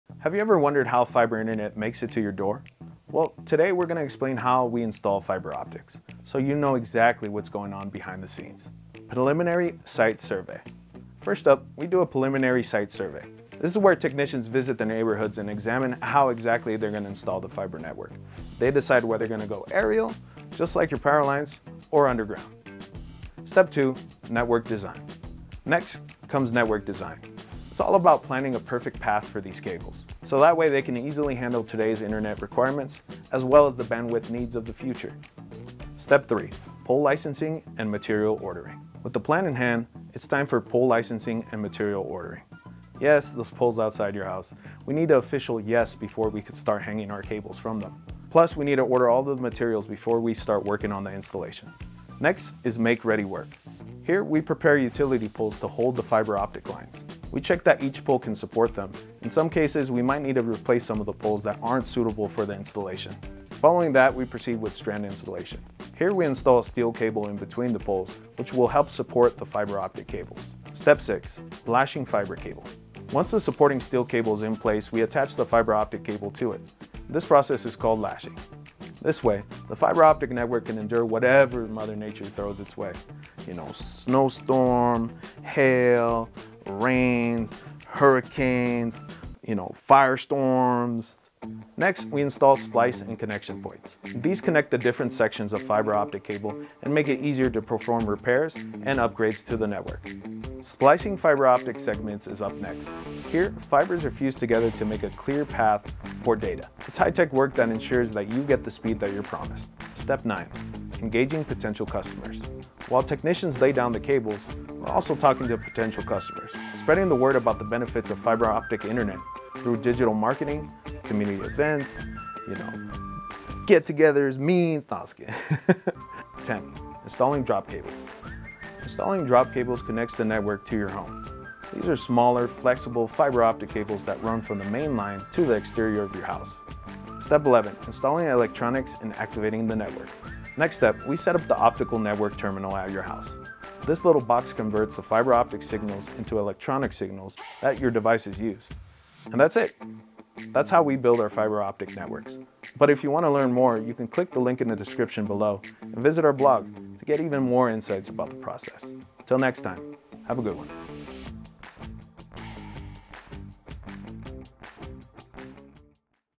Fiber Optic Installation, EXPLAINED - Race Communications
Have you ever wondered how you get high-speed internet to your home? Let’s break down the process of fiber optic installation into easy-to-understand steps so you’re well informed about what goes behind the scenes.